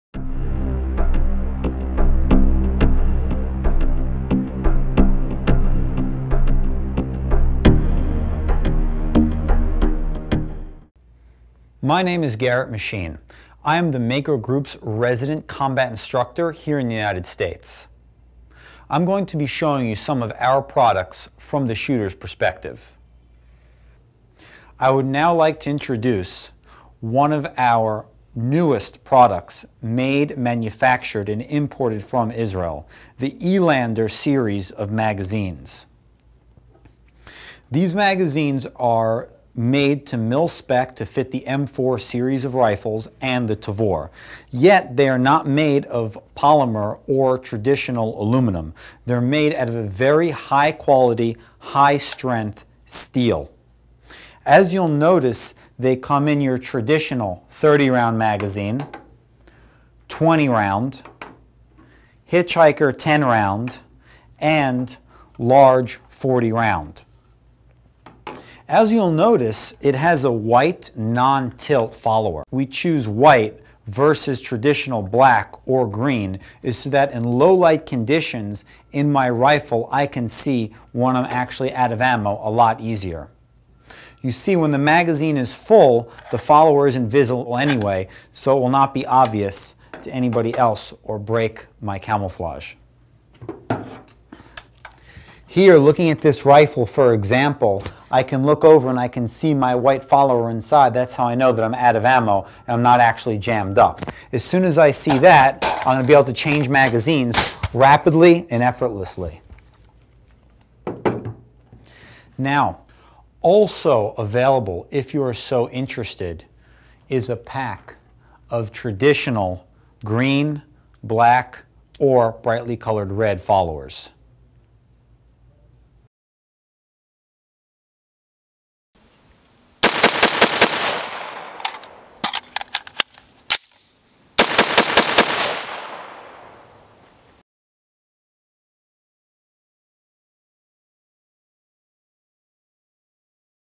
E-Lander Magazines from The Mako Group and Englander
E-LANDER MAGS are high-performance durable steel magazines.

Made in Israel for the Tavor, M16, and common sidearms, they were tested by special military forces and anti-terror units. These magazines have proven to withstand severe combat conditions and requirements.

Backed by over six decades of experience the company has specialized in the manufacturing of precision-stamped metal parts and tools with an uncompromising commitment to quality and reliability.